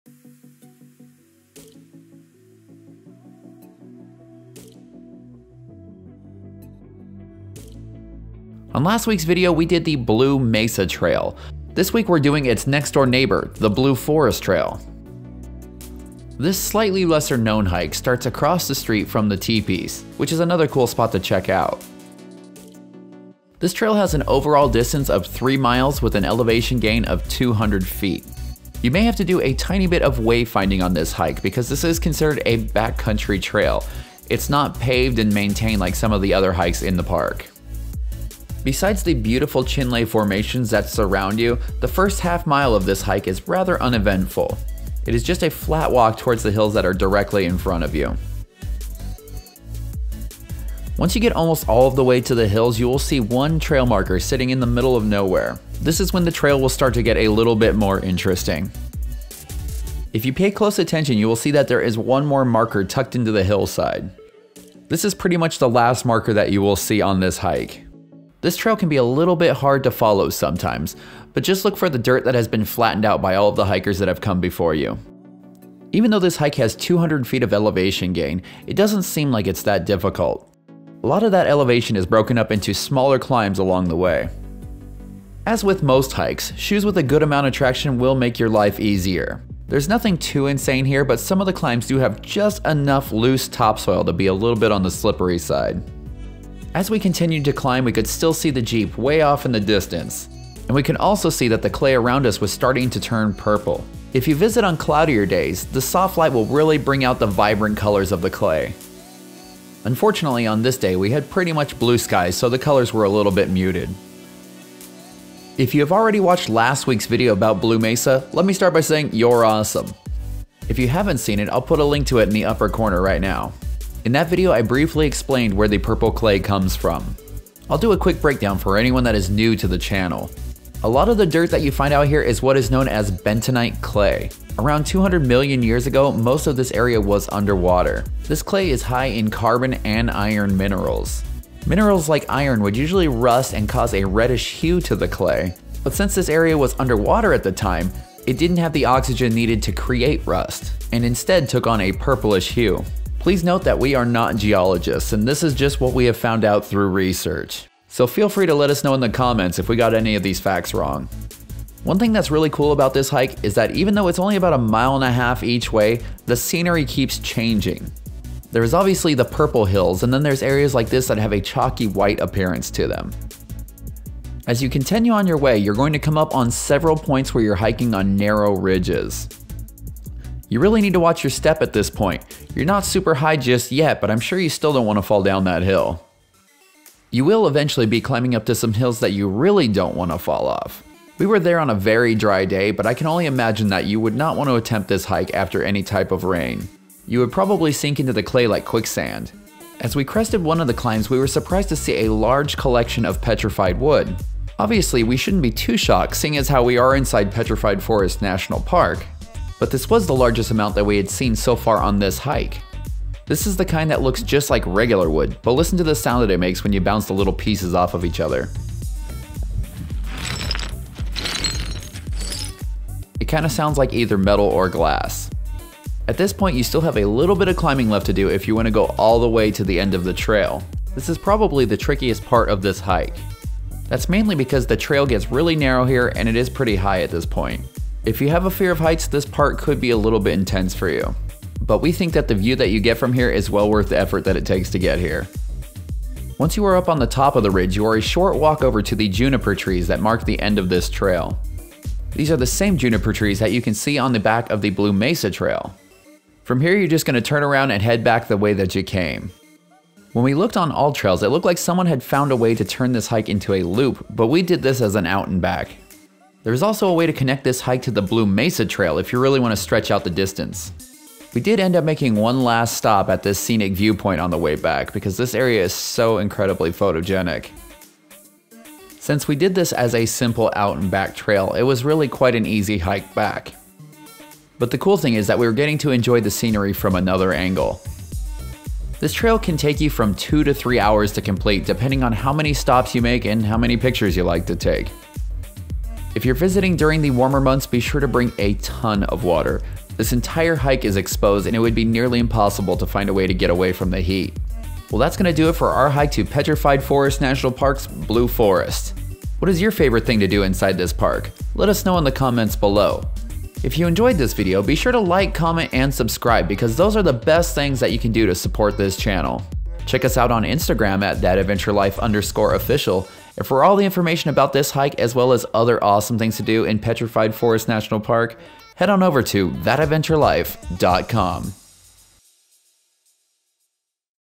Blue Forest Trail, One of the Best-Kept Secrets in Petrified Forest NP
It is surprising that Blue Forest Trail in Petrified Forest National Park in Arizona isn't considered to be one of its main attractions. Since it is a backcountry trail, not many people know about it. There is no paved trail and very minimal signage. However, if you're adventurous enough to attempt this hike, you'll be rewarded with an intimate view of the colorful hills of badland, petrified woods, and an unforgettable adventure. And if you prefer, you can also connect the Blue Forest trail with the beautiful, established Blue Mesa trail for a longer hike. Also, you will be exploring 3 million years of history just in this short hike.

𝐆𝐞𝐚𝐫 𝐮𝐬𝐞𝐝 𝐢𝐧 𝐭𝐡𝐢𝐬 𝐯𝐢𝐝𝐞𝐨:

GoPro HERO11 Black

GoPro HERO10 Black